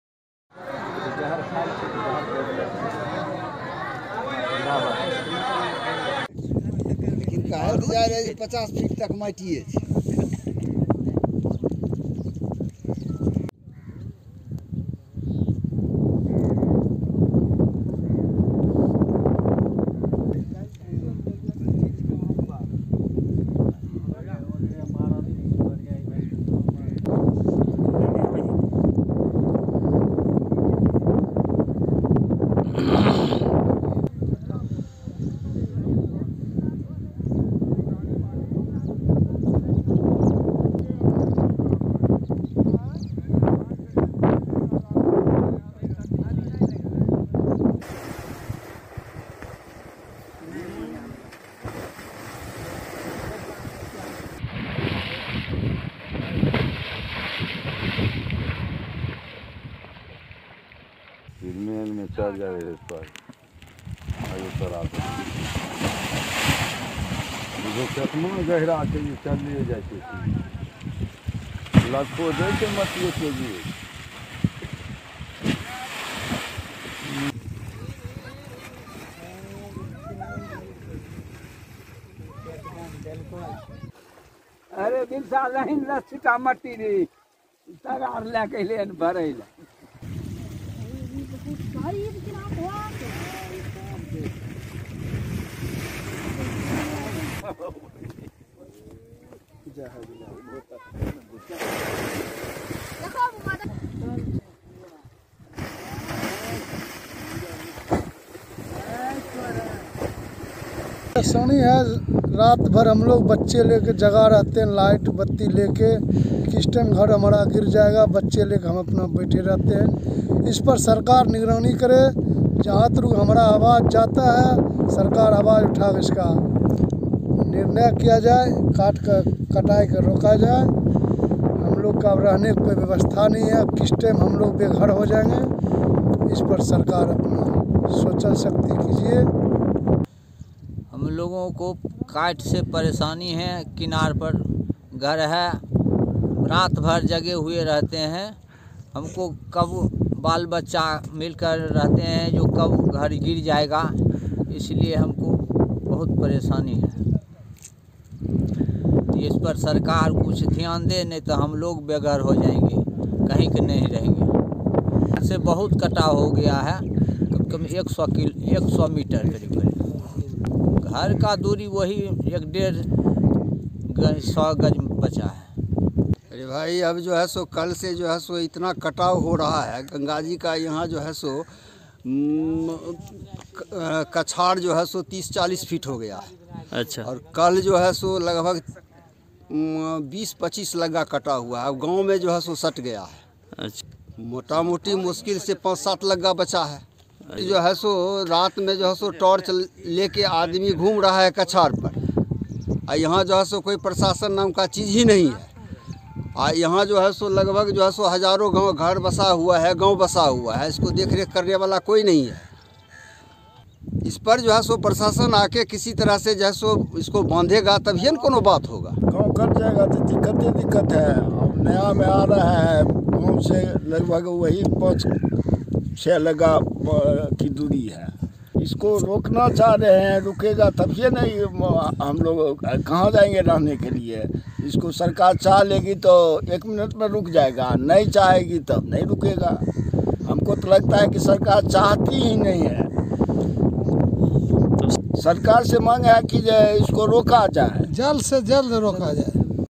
Begusarai में Ganga कटाव से घरों पर मंडराया खतरा, ग्रामीण बोले- हम लोग बेघर हो जाएंगे, Watch Video
बिहार के बेगूसराय में ठंड के बीच गंगा में तेज कटाव (Begusarai Ganga News) से ग्रामीणों में दहशत का माहौल बना हुआ है। मामला बलिया प्रखंड क्षेत्र के भवानंदपुर पंचायत के शिवनगर गांव के पास का है। गंगा नदी में हो रहे रोज कटाव से ग्रामीणों में त्राहिमाम मचा हुआ है। तस्वीरों में देख सकते हैं कि किस तरह गंगा किनारे कटाव हो रहा। आलम ये है कि ग्रामीण रातभर जाग हालात पर नजर रख रहे कि कहीं गंगा कटाव घर तक ना पहुंच जाए। ग्रामीणों का कहना है कि हम लोग बेघर हो जाएंगे... कहीं के नहीं रहेंगे।